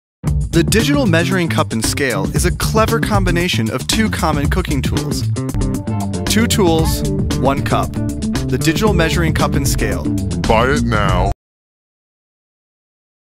DigiCup Digital Measuring Cup, Scale & Thermometer
🔥🔥 Are you looking for a measuring cup, thermometer and scale all in one? Well, you came to the right place!😍🤗
This ingenious idea of combining a measuring cup and scale in one is now a reality.
Not only does this Digital Measuring Cup Scale measures ingredients, but it also gives you a temperature reading of the ingredients. Perfect for those recipes that require liquids to be a certain temperature!